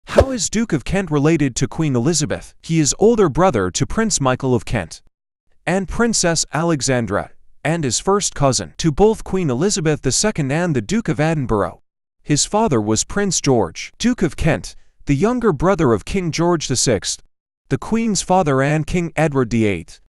How is Duke of Kent related to Queen Elizabeth?
He is older brother to Prince Michael of Kent and Princess Alexandra, and is first cousinr to both Queen Elizabeth II and The Duke of Edinburgh. His father was Prince George, Duke of Kent, the younger brother of King George VI (The Queen's father) and King Edward VIII.